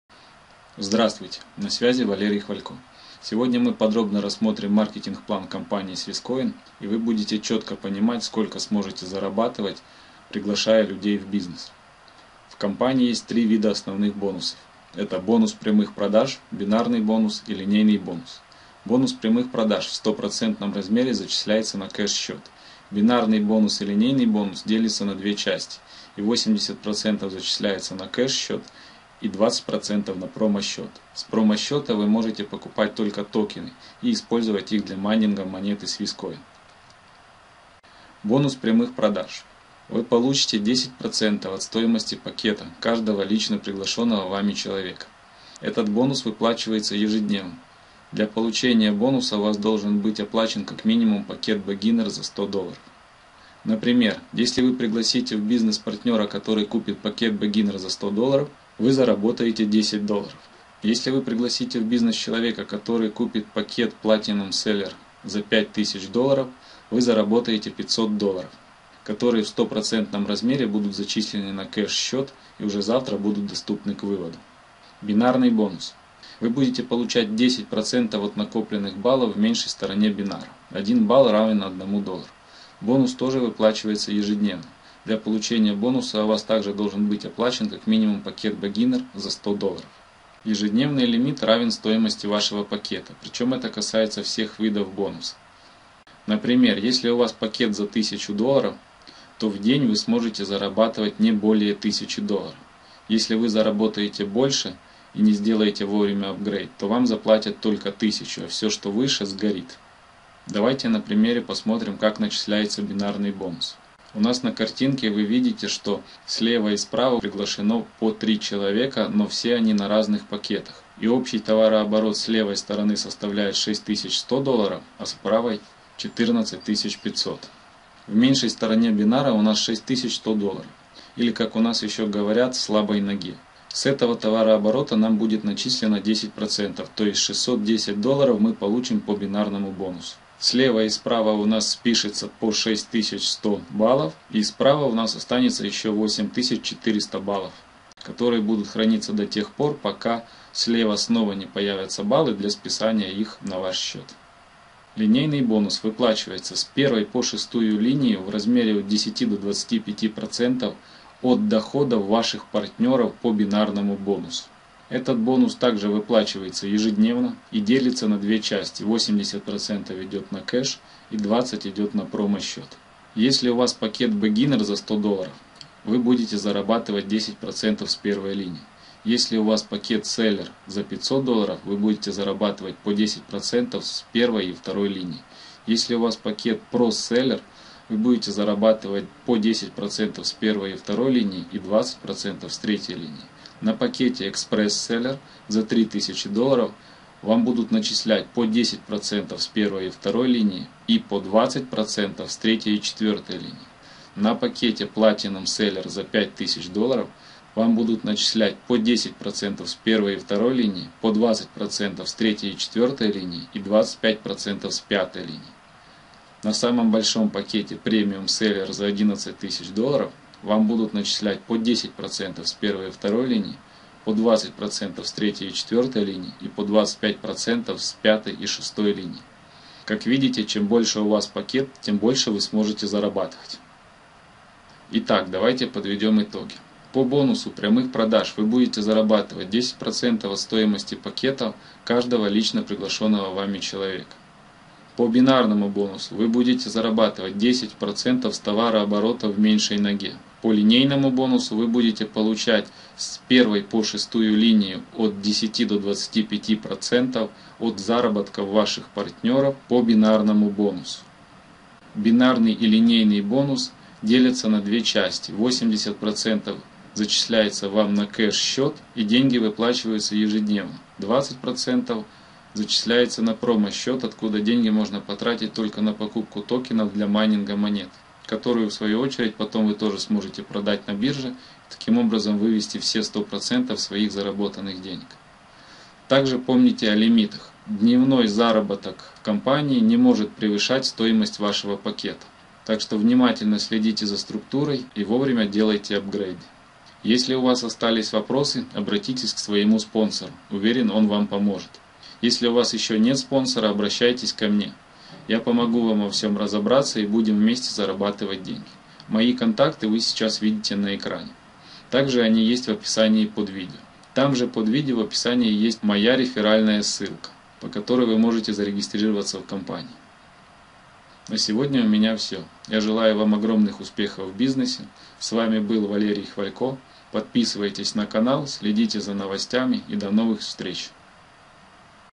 SwisCoin Маркетинг план подробно!
Криптовалюта Swisscoin представляет собой виртуальную валюту, основанную на новейших технологиях и защищенную криптографией. Ни одно правительство не имеет над ней контроля. Наиболее известная криптовалюта – Биткоин. Ее рассматривают как денежную валюту, используемую внутри системы.

Сейчас Bitcoin является современной цифровой валютой, которую принимают многие интернет-магазины. Стоит выделить удобство открытия счета. Поскольку не возникает никаких сложностей, все больше людей в развитых странах обращают внимание на цифровую валюту Биткоин, пользуются POS-терминалами, банкоматами, аппаратными кошельками. В Африке, Азии Биткоин заменяет банковское обслуживание, которое обходится дороже и доступно не всем людям.

С уважением, Арман.